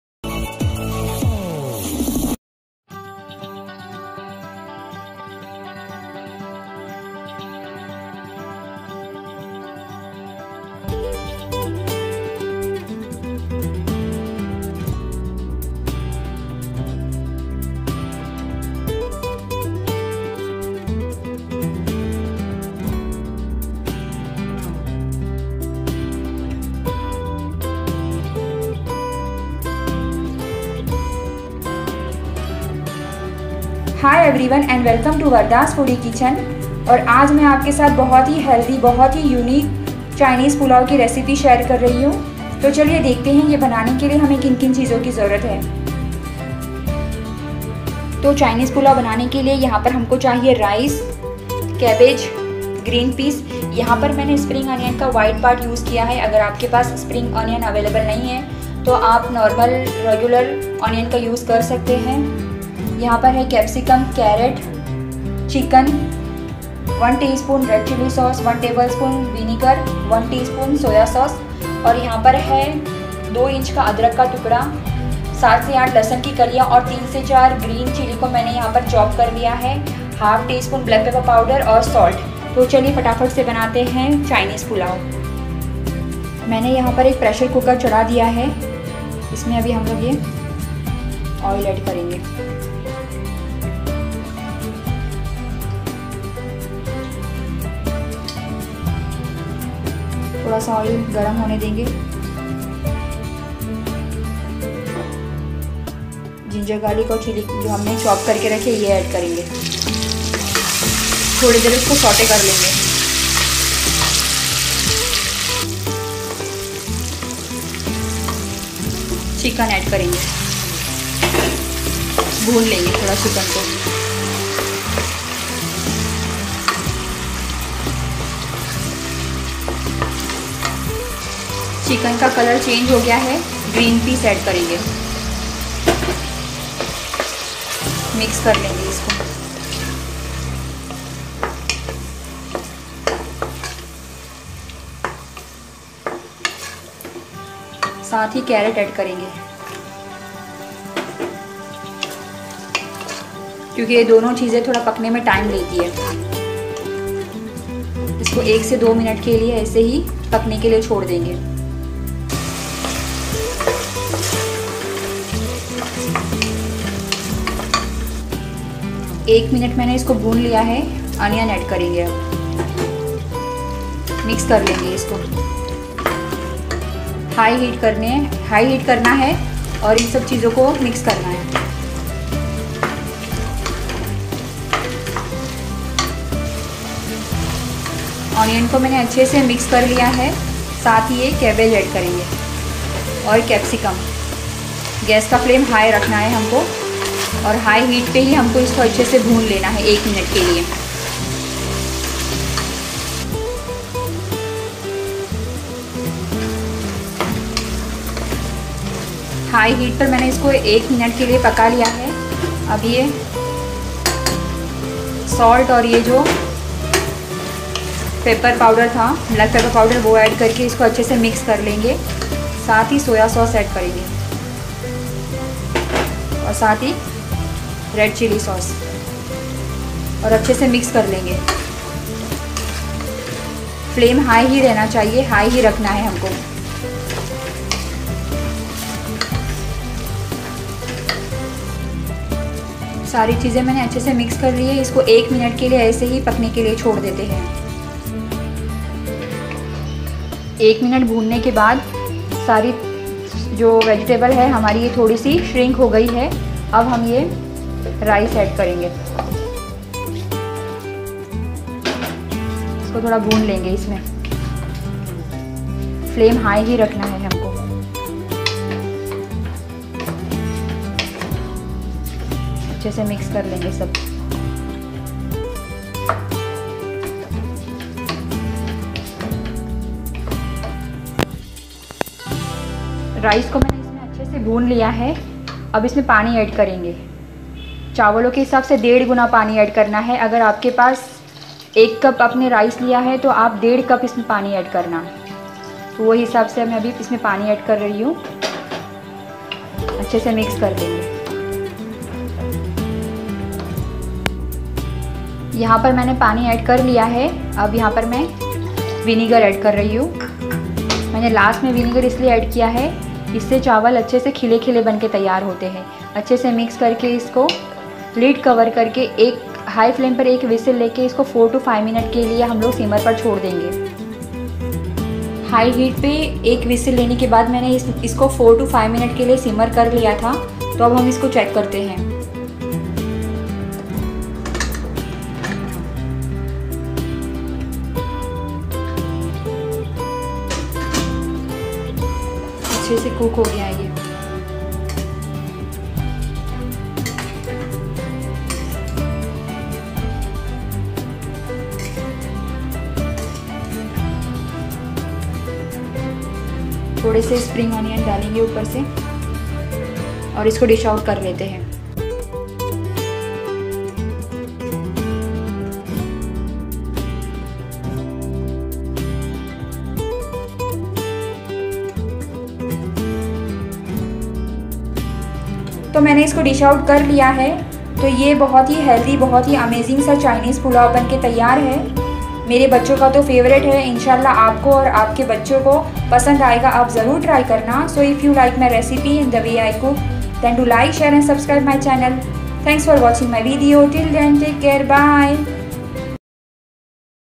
Chinese Pulao ||Chinese Pulao Recipe || Chinese Pulao in Pressure Cooker|Wardah's Foodie Kitchen
Hi Everyone

Assalam-o- alaikum

Welcome to
Wardah's Foodie Kitchen

Today I am going to share recipe of How To Make Yummy & Tasty Chicken Chinese Pulao in Pressure Cooker

Do try & share your experience with us in the Comments section